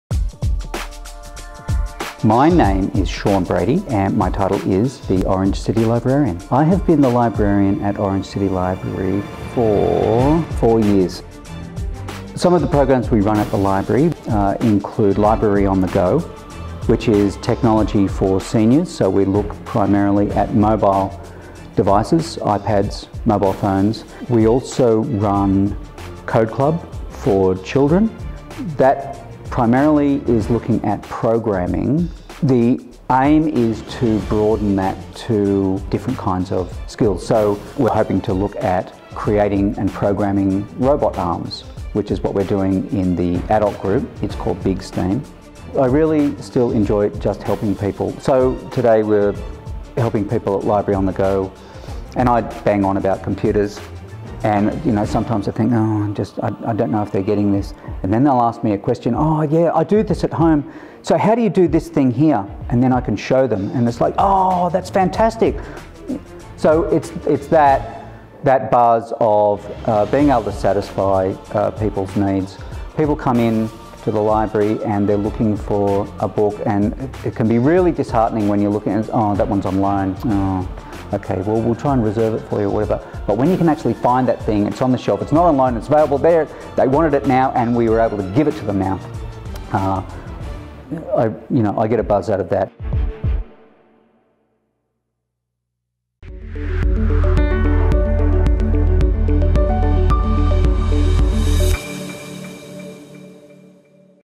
Behind the scenes at Orange City Library
There's so much more to the library than books and audio cassettes.
Sean Brady, Orange City Librarian, is doing something different every day.
Whether it's programming robot arms, building cardboard forts, teaching technology to seniors or answering every one of life's little mysteries. Sean does it all and he loves his job.
Check out the latest installment in our Behind the Scenes videos.